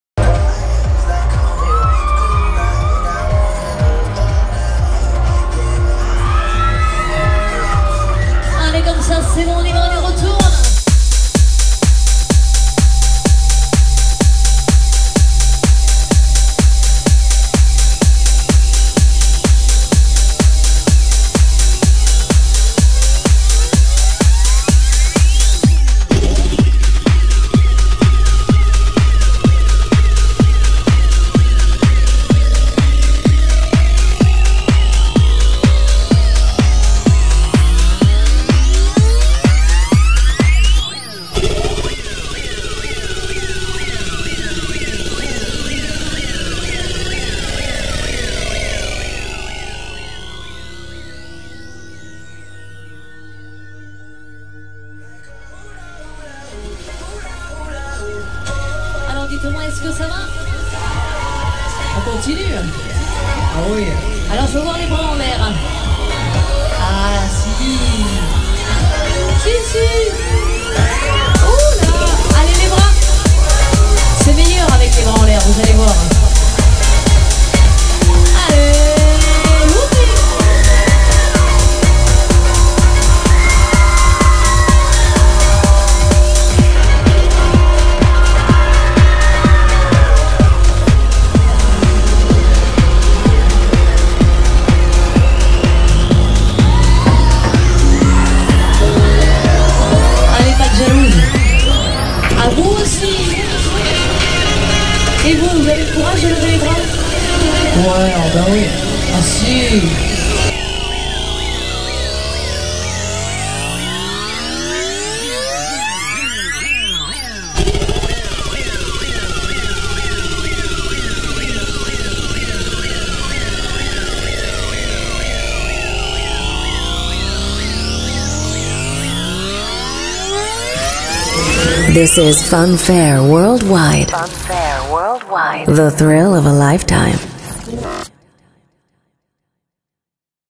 "SPINBALL" (CARPENTIER) THRILL LOOPING RIDE
Manufacturer: Van der Beek
Type: Spinball
Owner: Carpentier